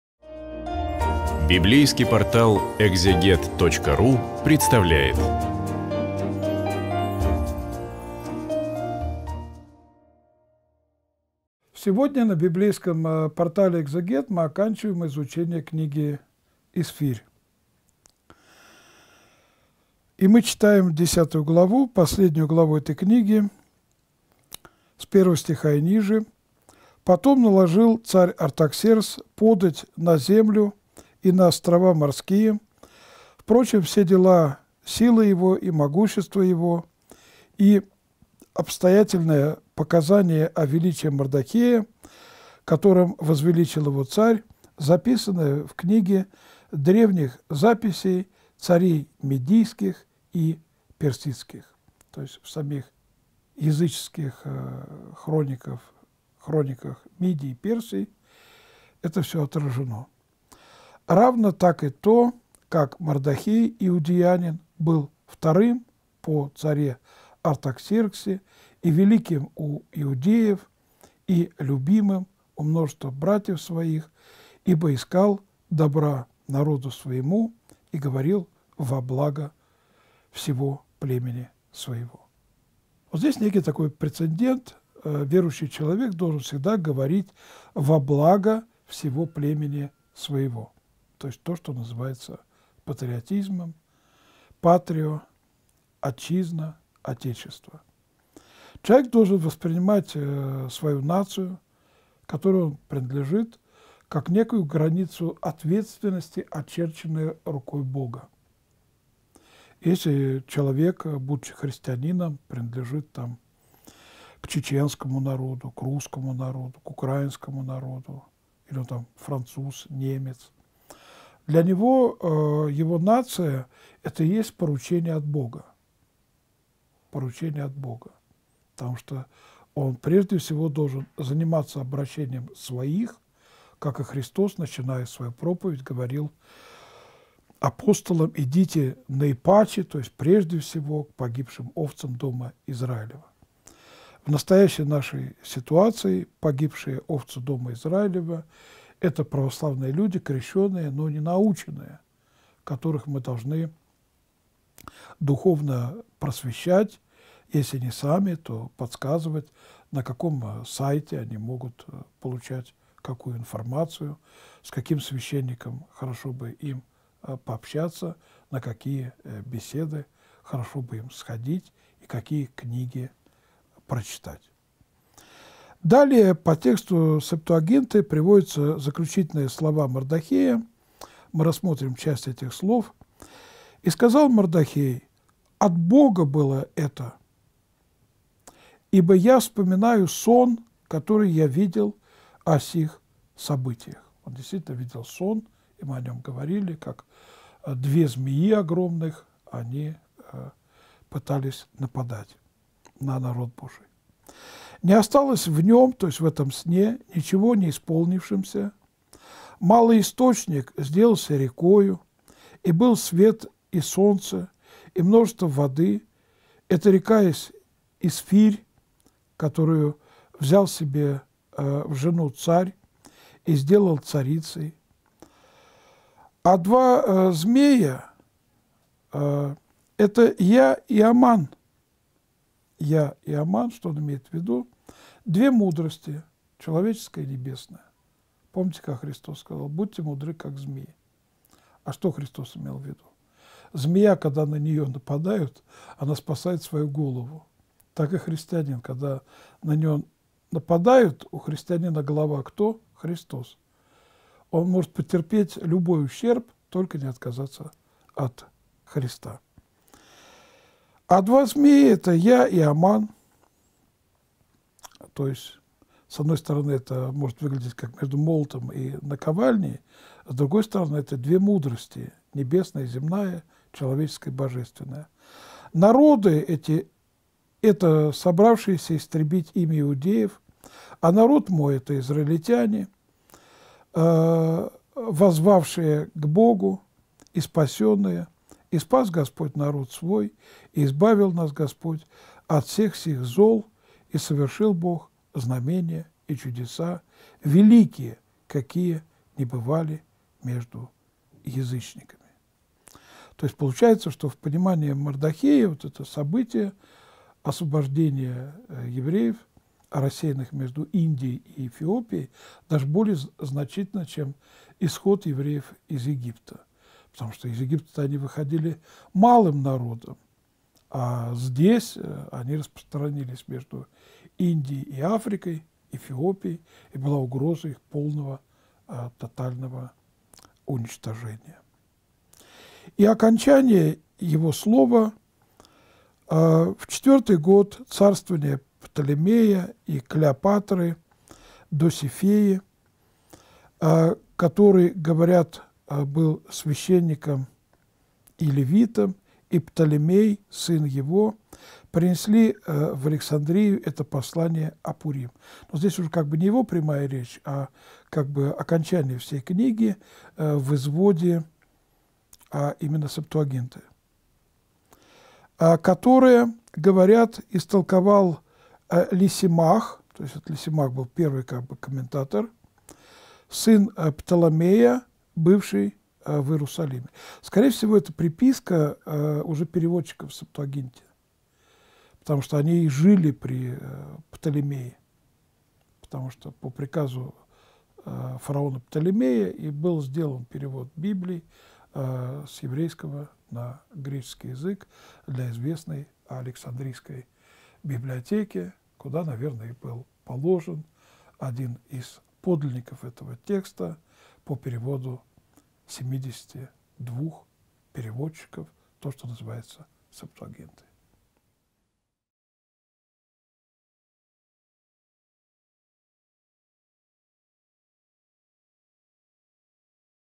Книга Есфири. Глава 10. Протоиерей Олег Стеняев. Ветхий Завет
Последнее глава книги Есфирь показывает могущество Артаксеркса и величие Мардохея. Мардохей вспоминает свой сон, который целиком исполнился. Рассмотрение вопроса о нации, как о границе ответственности.
Протоиерей Олег Стеняев — проповедник, миссионер, публицист, автор нескольких книг. Специализируется в области сектоведения и сравнительного богословия.